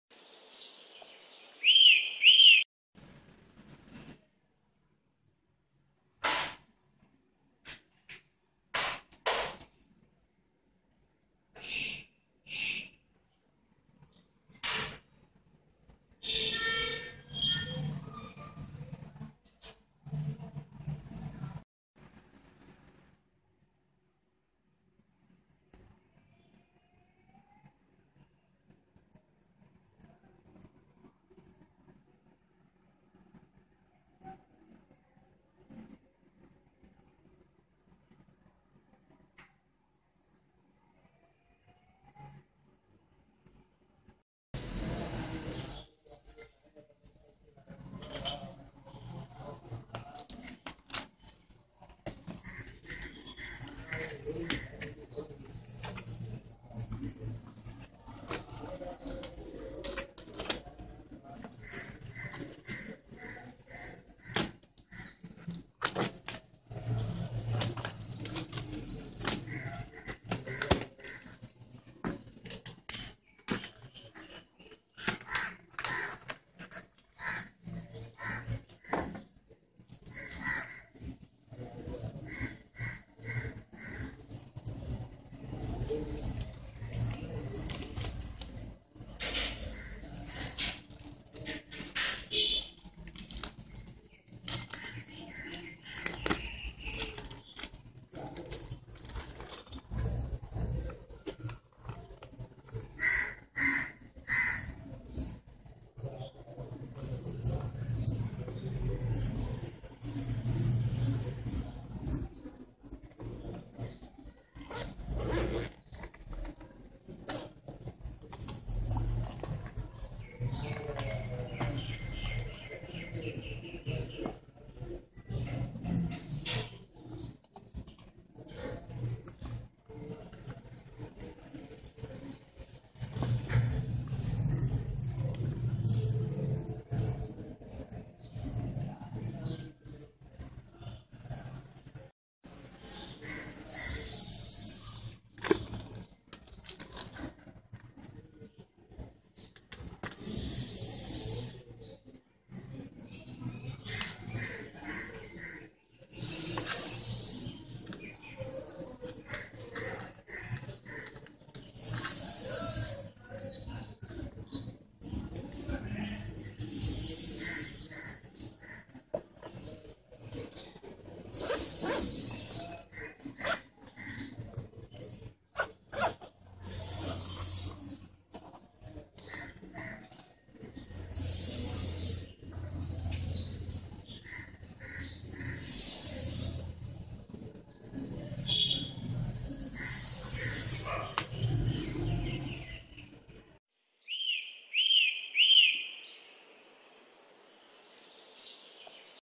Hard Disk 2.5 Inch Case Airplus Review
Airplus Pocket Drive Pouch 2.5 inch External Hard Disk Cover  (For Sony,Hitachi, iomega, Toshiba, Dell, Lenovo, HP,, Black)

Type: External Hard Disk Cover
Designed For: Sony,Hitachi, iomega, Toshiba, Dell, Lenovo, HP,
Water Resistant
Shock Resistant